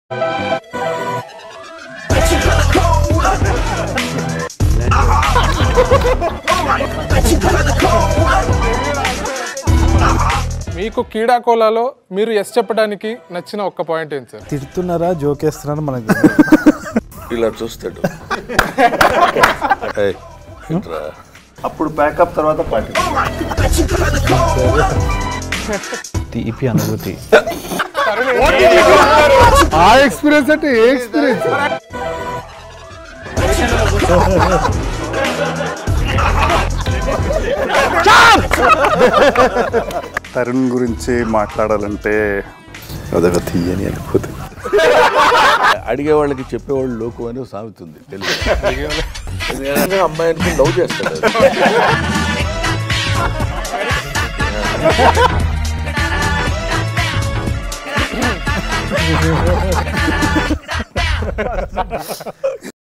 The Fun & Candid Keedaa Cola Cast Interview Promo | Tharun Bhascker | Brahmanandam Garu | VG Sainma
Get ready for a wild ride with the OG Brahmanandam garu, Tharun Bhascker & the entire ‘KeedaaCola' gang! The full interview coming out tomorrow!

IN THEATRES NOV 3